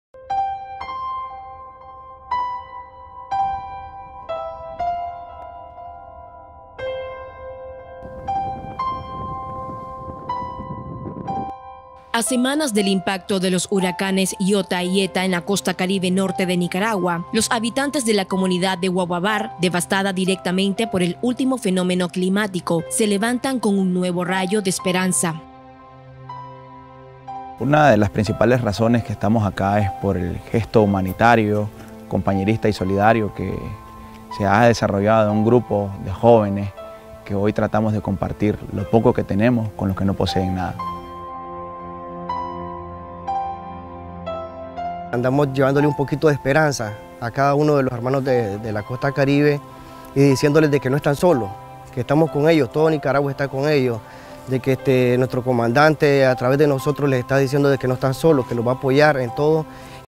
Nuestro Buen Gobierno es la esperanza para los afectados por el paso de Iota y Eta
Nuestro buen gobierno presidido por el Comandante Daniel Ortega y la Compañera Rosario Murillo, garantizan que las familias afectadas por Iota y Eta, hoy tengan una esperanza para mejorar sus viviendas y su producción agrícola.